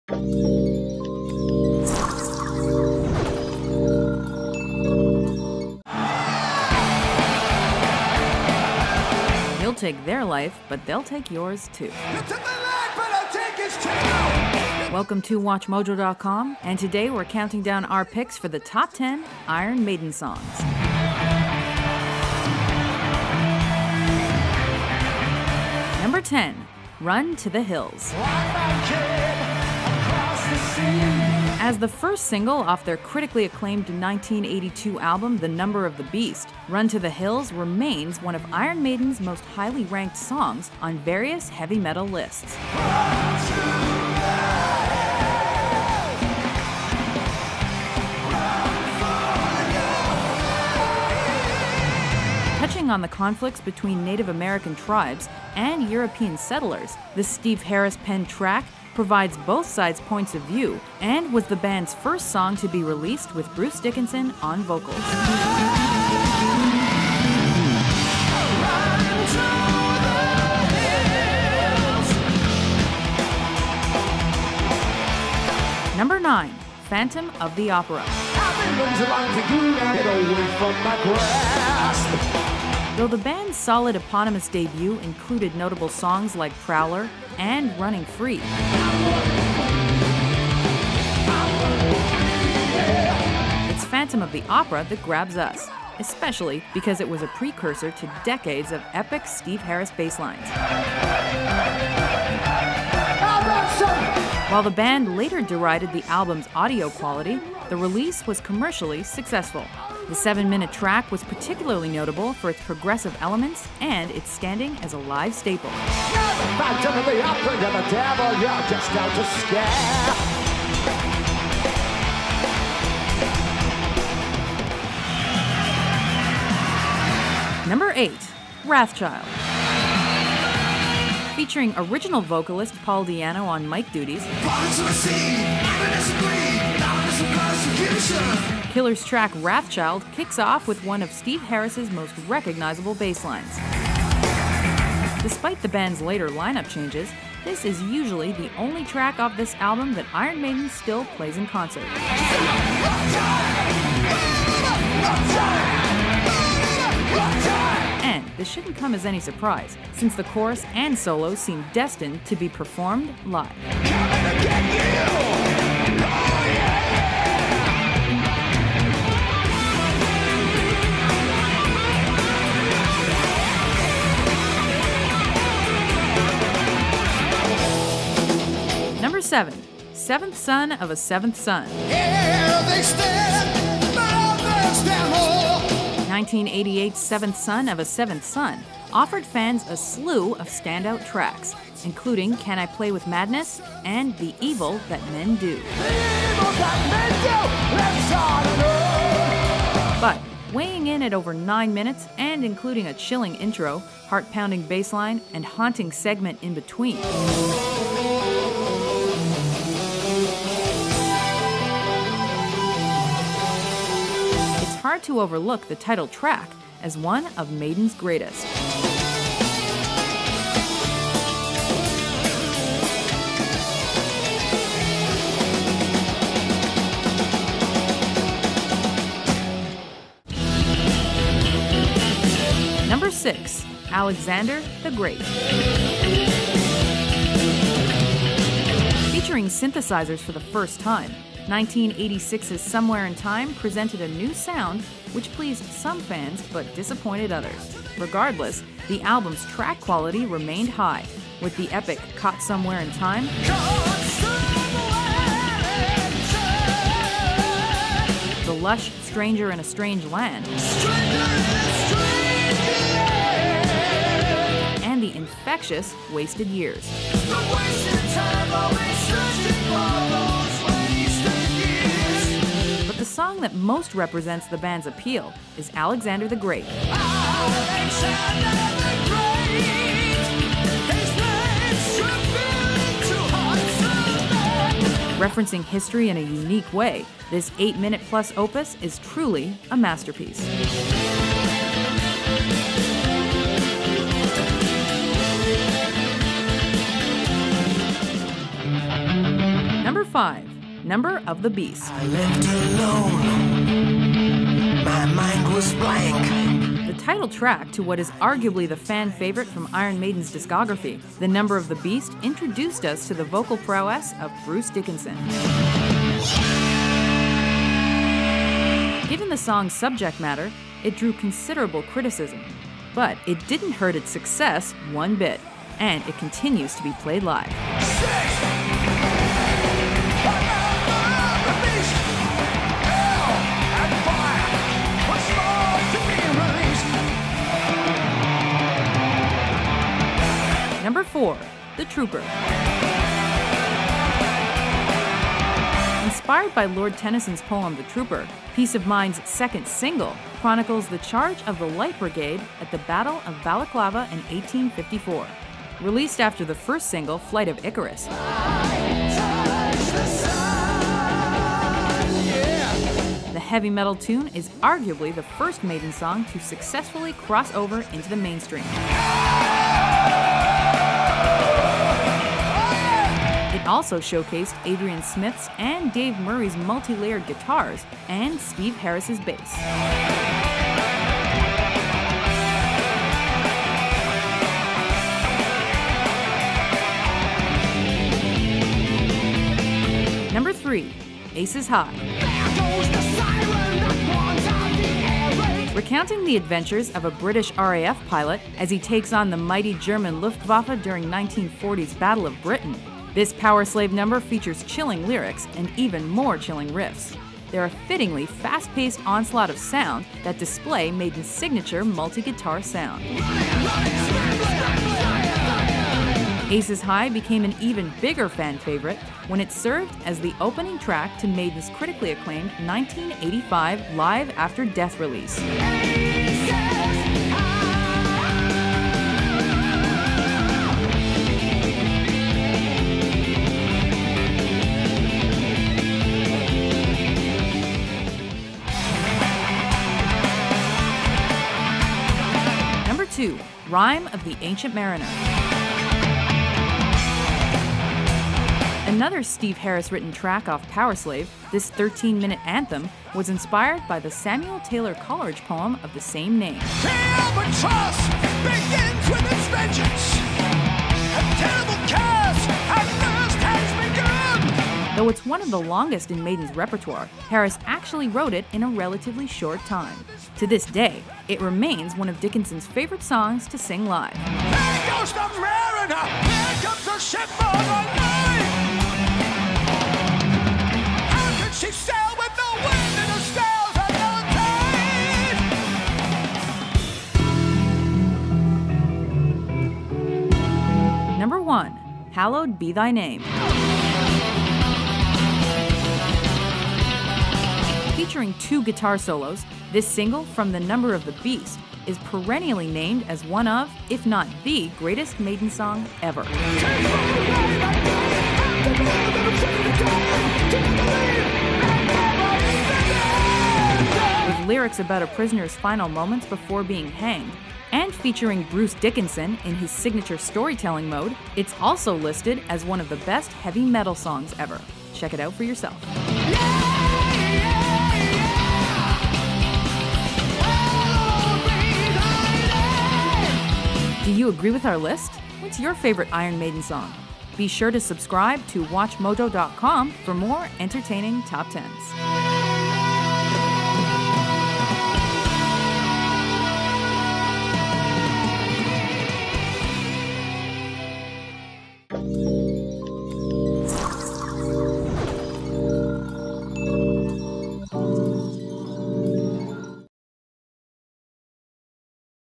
Top 10 Iron Maiden Songs
You'll take their life, but they'll take yours, too.  Join WatchMojo.com as we count down our picks for the top 10 Iron Maiden songs.  Special thanks to our users "aldqbigsquare", "maxdap1mp92", "akt" and "Andres Orozco" for submitting the idea for this video at WatchMojo.com/suggest :)